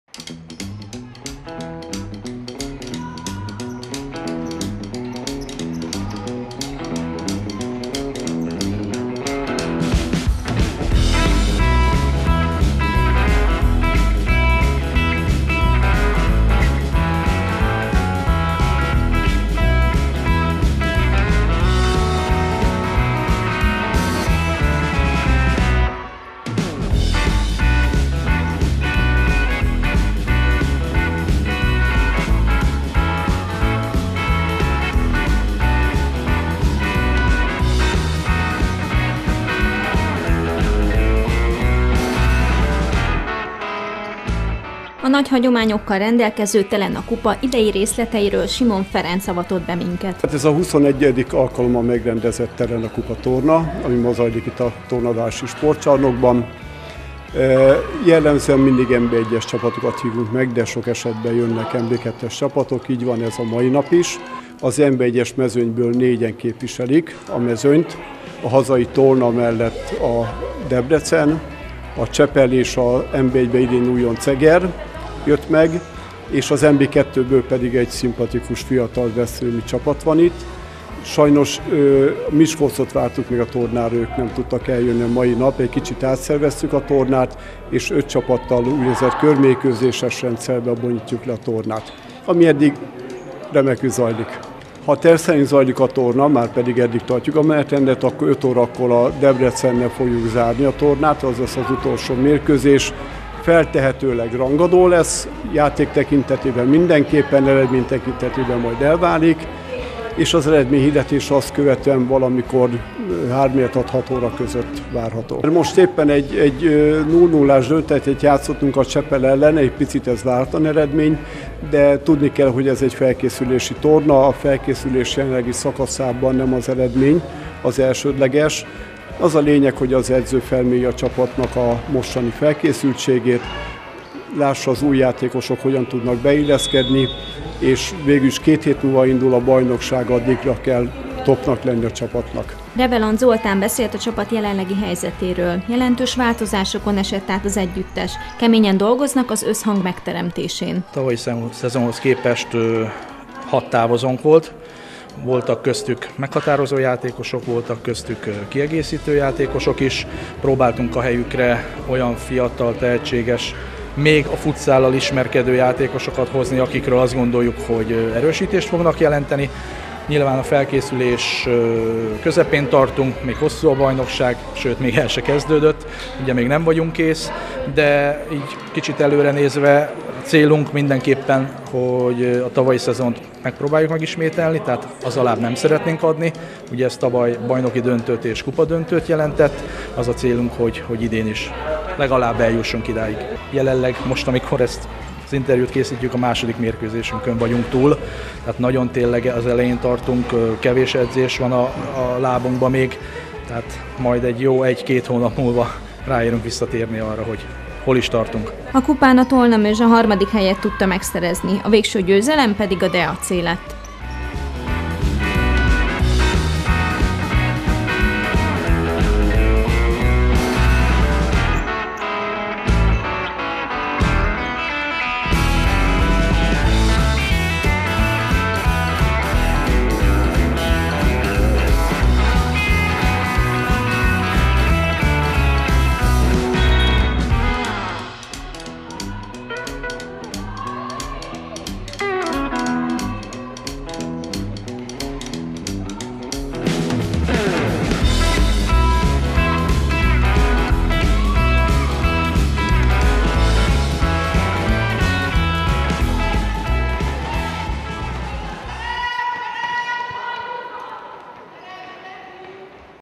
Harmadik helyet szerezték meg a kupán - a szezonra hangolódtak a Thelena kupán a futsálosok
Izgatottan várta Reveland Zoltán és csapata a Thelena kupát, ugyanis a bajnokság kezdete előtt ez remek alkalom, hogy képet kapjanak a csapat jelenlegi helyzetéről. Tolna-Mözsön 21.x rendezték meg a felkészülés jegyében zajló kupát.